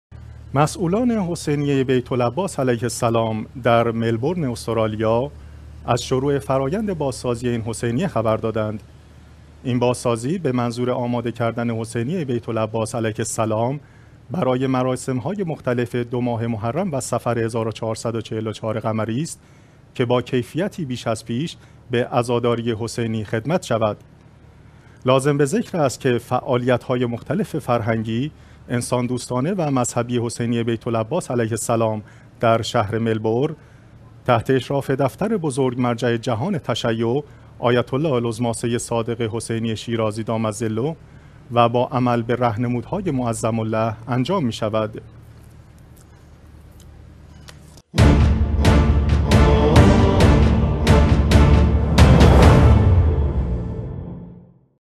بازسازی حسینیه بیت العباس در استرالیا
مسئولان حسینیه بیت العباس علیه السلام ، در ملبورن استرالیا از شروع فرایند بازسازی این حسینیه خبر دادند.
این بازسازی به منظور آماده کردن حسینیه‌ بیت العباس علیه السلام برای مراسم‌های مختلف دو‌ماه محرم و صفر ۱۴۴۴ قمری است که با کیفیتی بیش از پیش به عزاداران حسینی خدمت شود.

لازم به ذکر است که فعالیت‌های مختلف فرهنگی، انسان‌دوستانه و مذهبی حسینیه بیت العباس علیه السلام در شهر ملبورن تحت اشراف دفتر بزرگ مرجع جهان تشیع ، آیت الله العظمی سید صادق حسینی شیرازی دام ظله و با عمل به رهنمودهای معظم له انجام می‌شود.